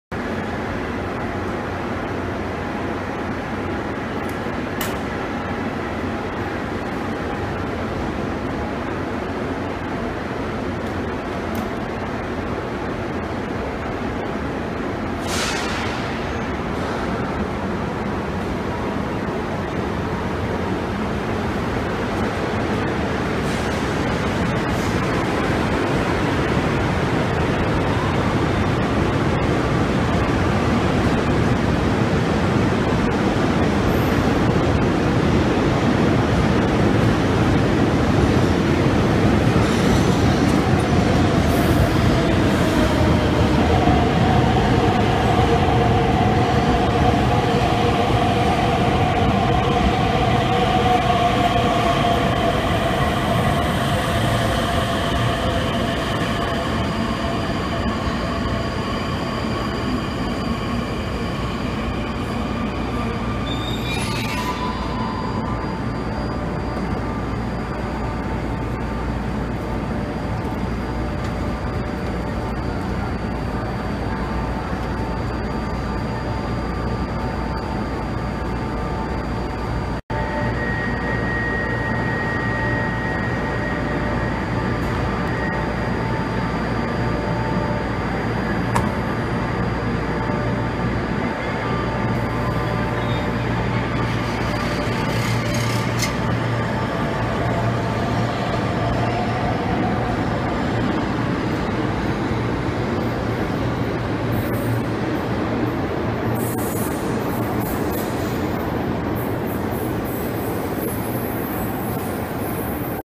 Metrô Rio - Trens em General Osório II (HD)
Metrô Rio linha 1/4 - Trens (Alstom/Mafersa e CNR 4000) na estação Ipanema/General Osório II.

OBS.:
Alstom/Mafersa sentido estação Uruguai;
CNR série 4000 sentido estação Jardim Oceânico.